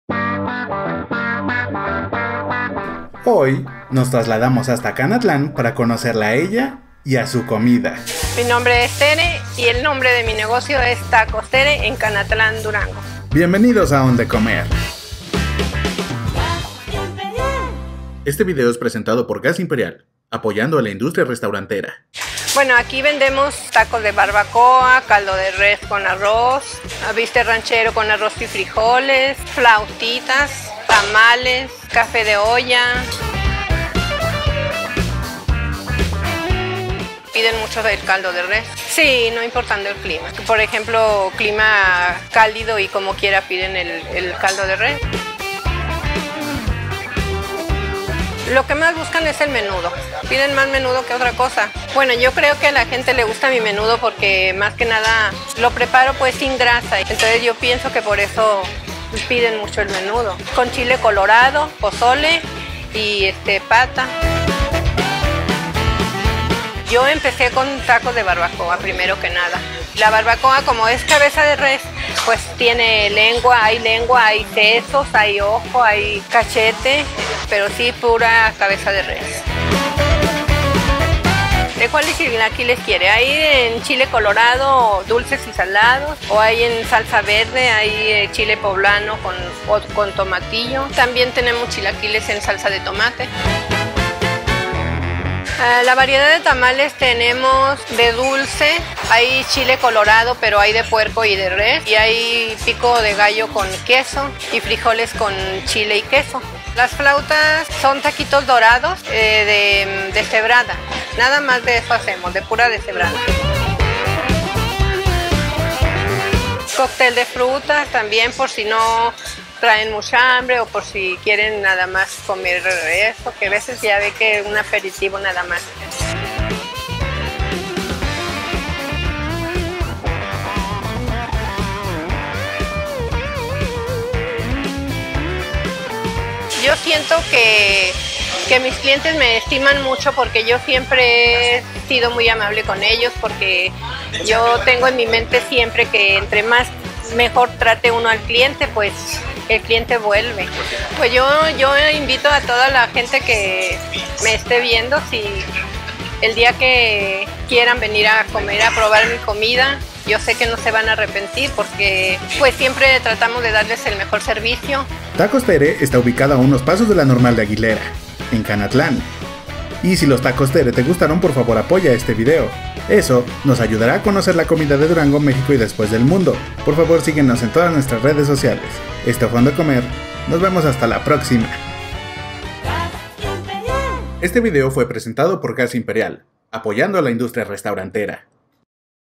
Tacos Tere (Canatlan, Dgo.) | Onde Comer Durango
Tragonautas nos fuimos hasta Canatlan para disfrutar el menudo, los tacos de barbacoa y el cocido de res en este emblemático lugar llamado Tacos Tere

Agradecemos a Gas Imperial Durango por su apoyo para realización de este video.

CONOCE MÁS:
🏠 Dirección: Carretera a Parral entronque a Santiago Papasquiaro
⏰ Horarios: Lunes a Sábado 6:00 am a 6:00 pm y Domingo de 6:00 am a 3:00 pm
💳 Pagos: Efectivo • Tarjeta
🥙 Opción Vegetariana: Si
🍺 Bebidas Alcohólicas: No
🌭 Menú Infantil: Si